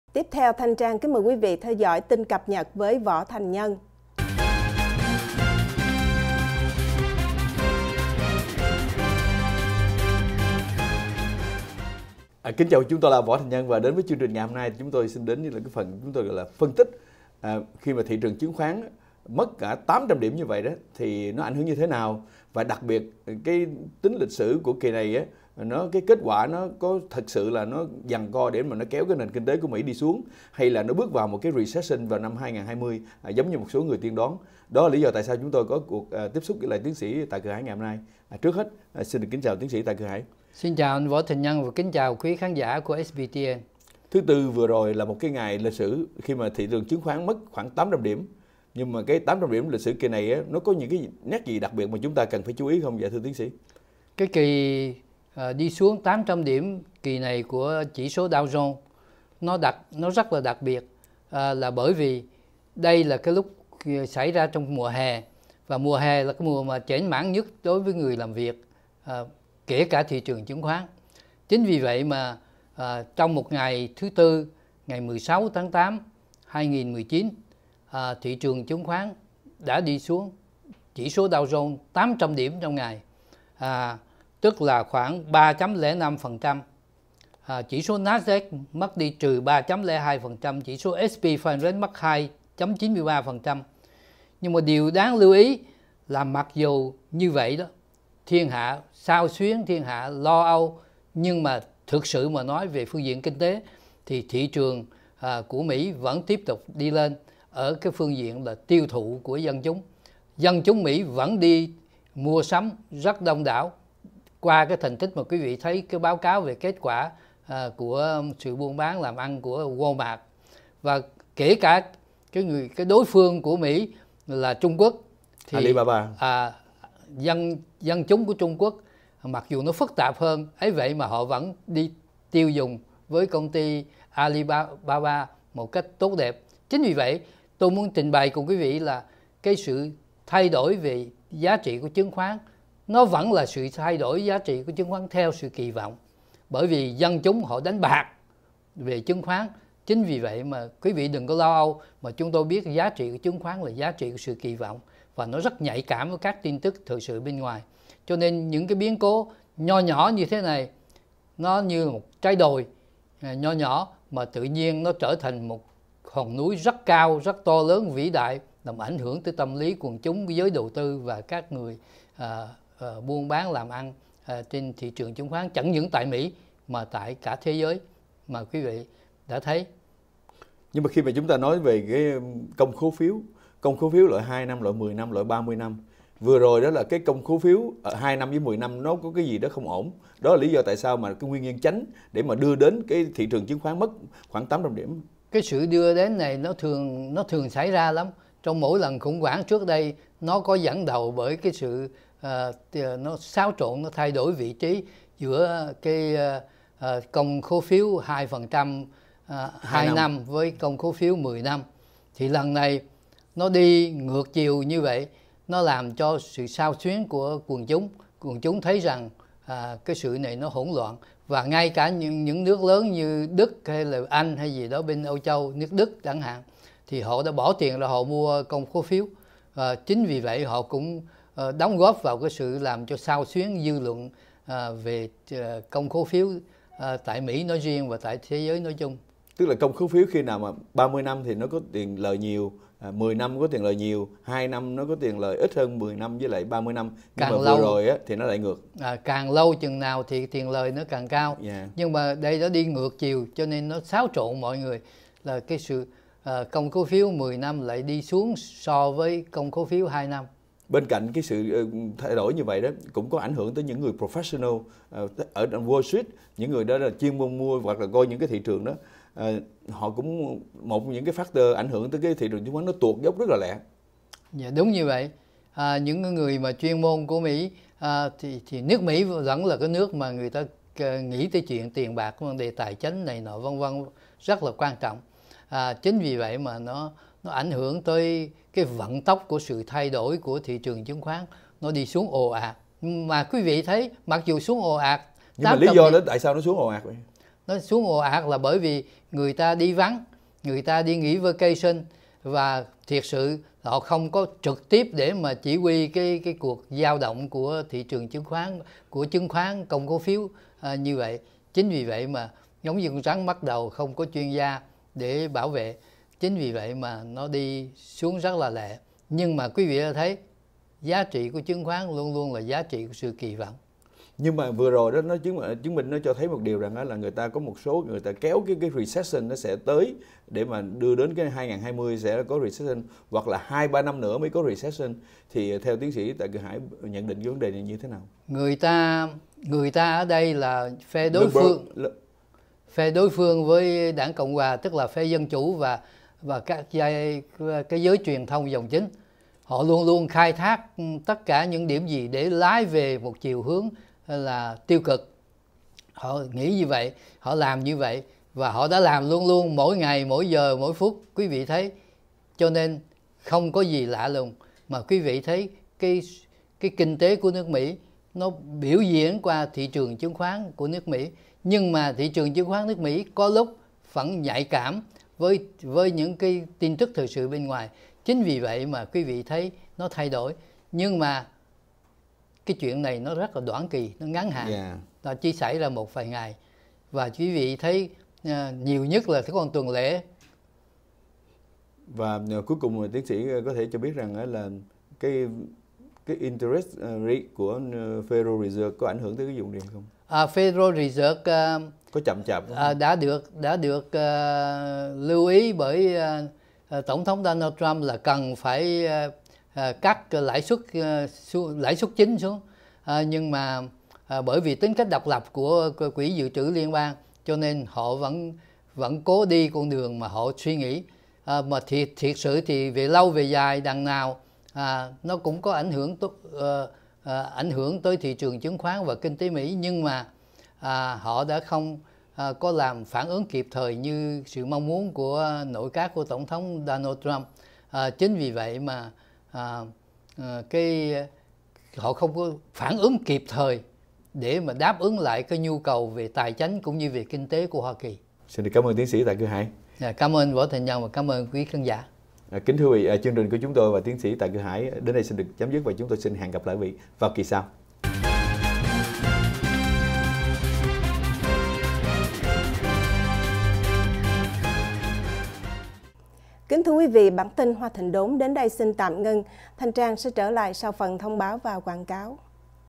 VATV | 4/8/2019 Chỉ Số Dow Jones Rớt 800 Điểm Ảnh Hưởng Của Thị Trường Chứng Khoán Ra Sao?
Tin Cập Nhật
19/08/2019
Tiến Sĩ Tạ Cự Hải
Võ Thành Nhân